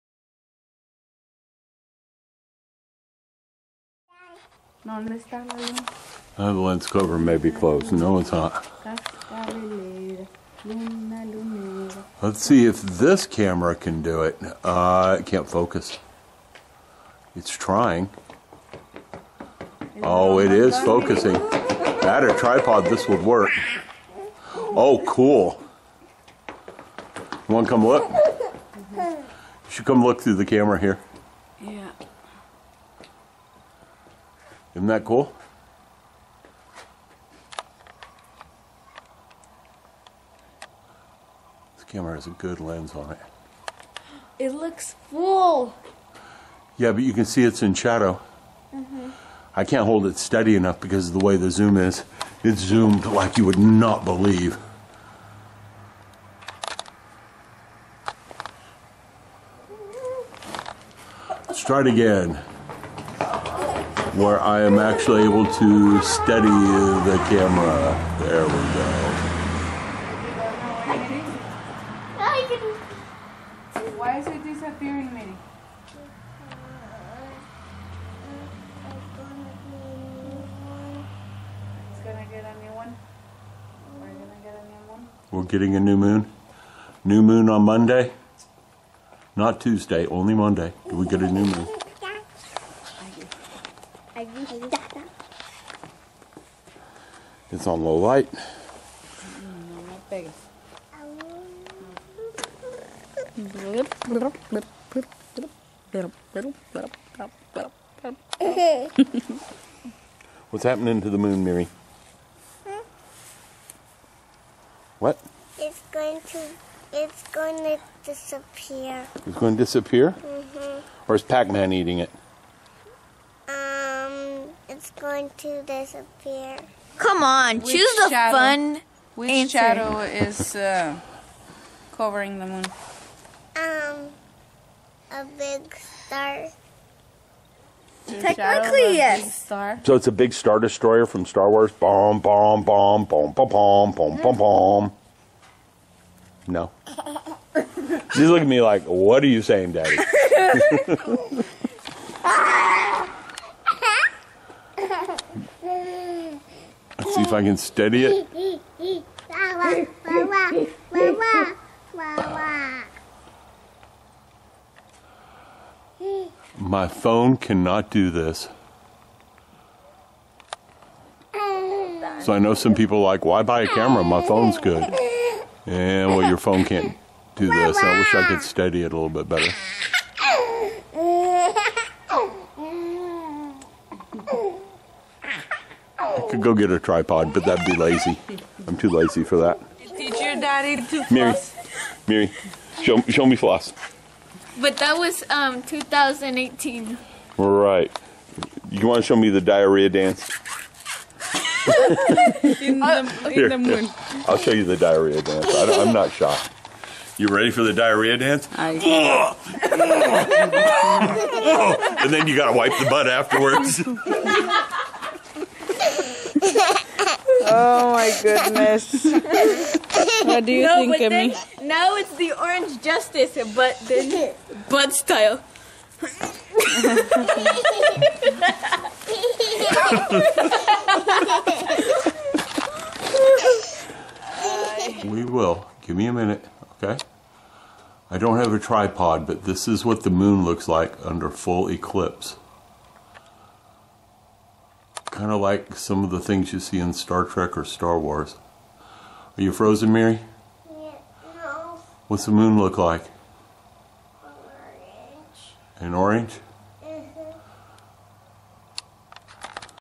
Blood Moon and the Diarrhea dance
Just some video shot of the Blood moon and the family goofing around.